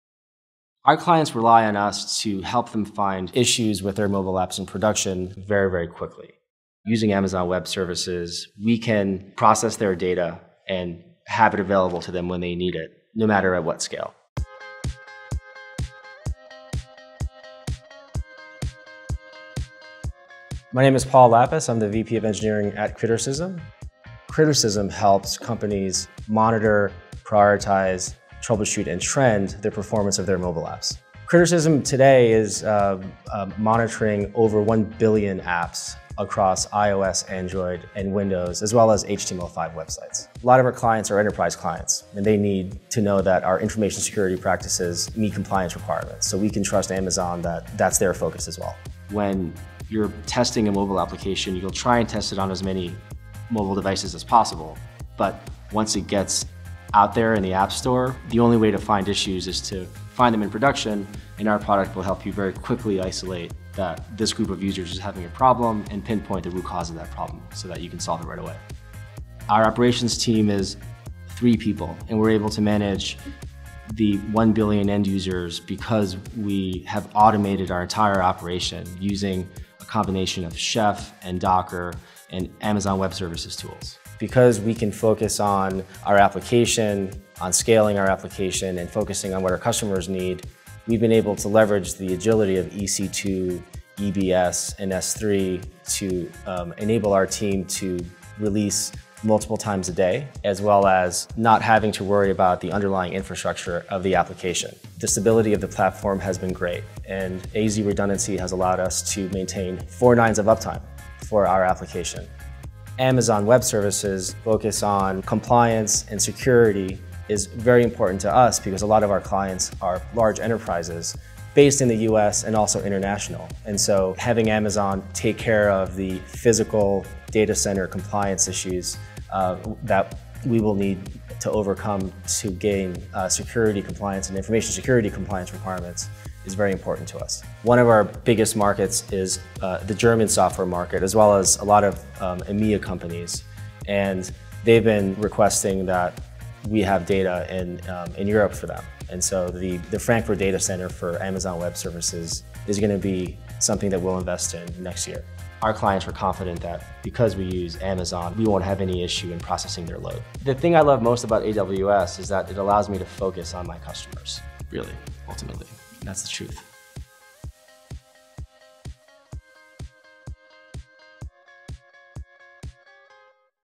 Crittercism Monitors Over 1 Billion Mobile Apps Using AWS
Crittercism helps companies accelerate their mobile business with mobile application performance monitoring services. The company leverages AWS to automate complex processes and scale to monitor over 1 billion mobile apps while meeting information security and compliance requirements. Learn about mobile app development on AWS